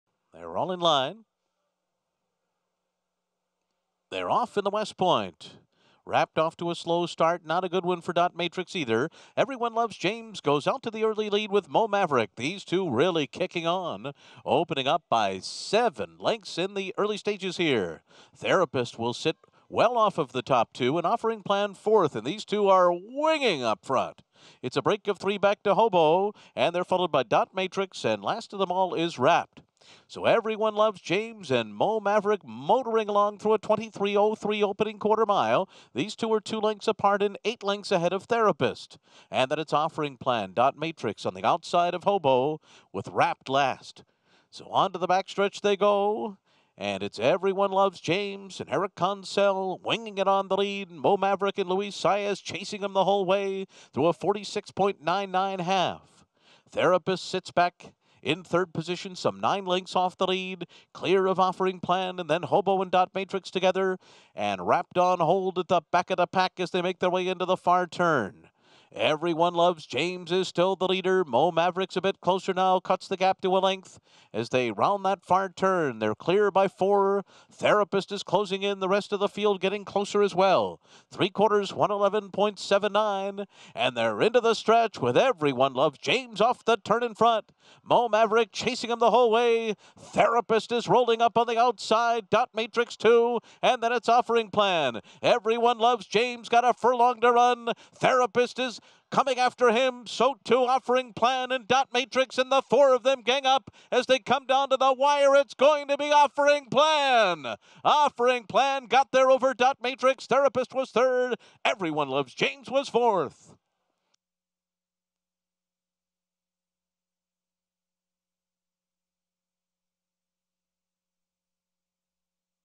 Offering Plan - 2019 - The West Point Stakes presented by Trustco Bank
Owned by Michael Dubb, Nantucket Thoroughbred Partners, Bethlehem Stables, and G. Aisquith, Offering Plan surged between rivals in deep stretch under Castellano to capture the $150,000 West Point presented by Trustco Bank.